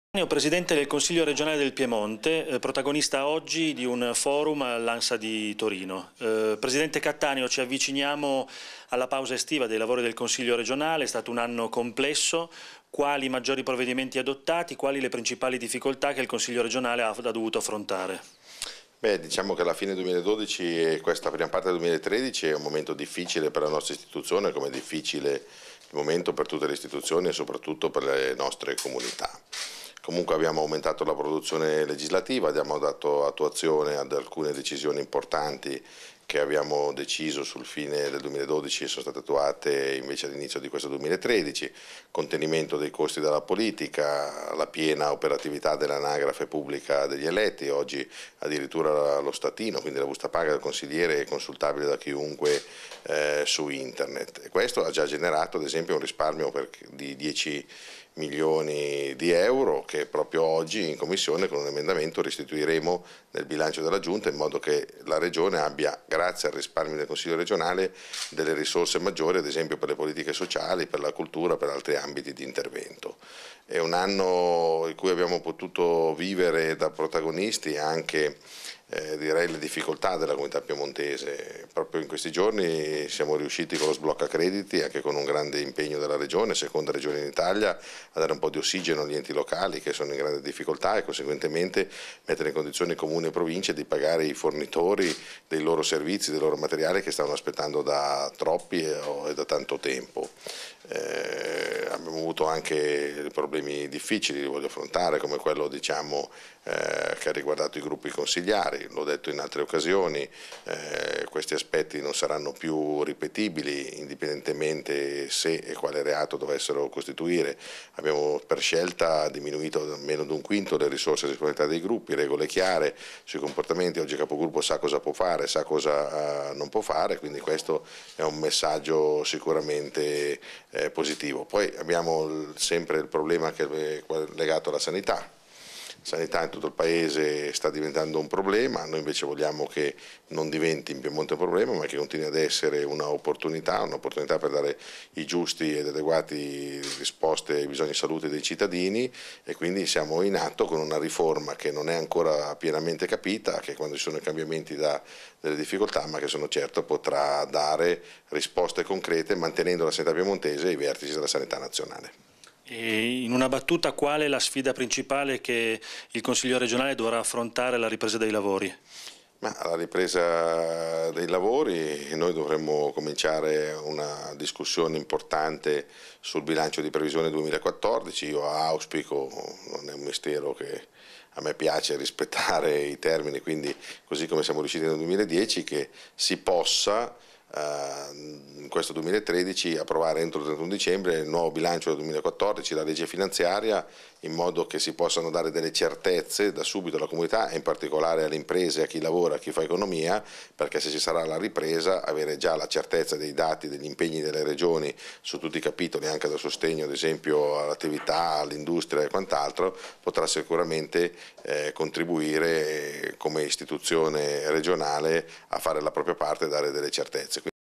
Un anno in Consiglio regionale
Il presidente del Consiglio regionale del Piemonte, protagonista di un forum all'Ansa di Torino